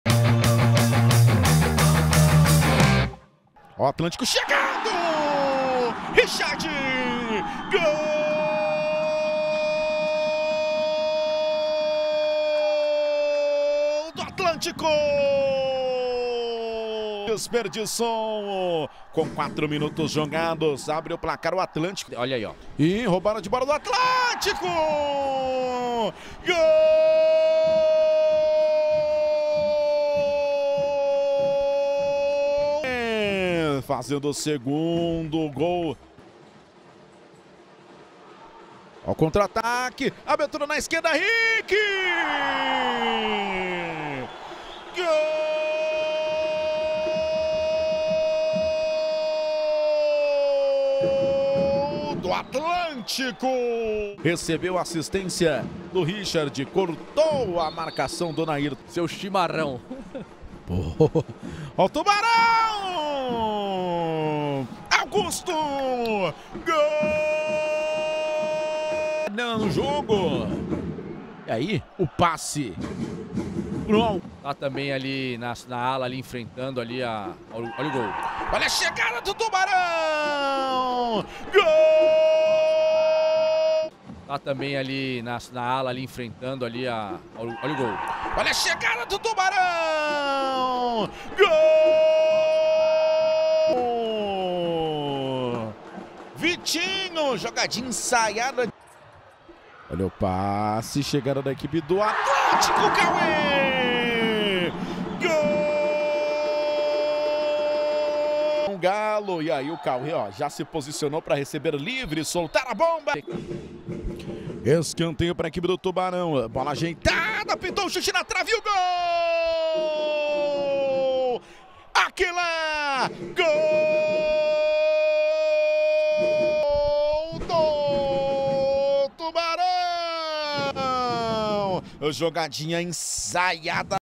LNF MRJACK.BET 2023 - 22ª RODADA - GOLS - ATL 4 X 3 TUB
GOLS:
ATLÂNTICO - RICHARD, RICK E KAUÊ (2)
TUBARÃO - AUGUSTO, VITINHO E ÁQUILA
TRANSMISSÃO: LNFTV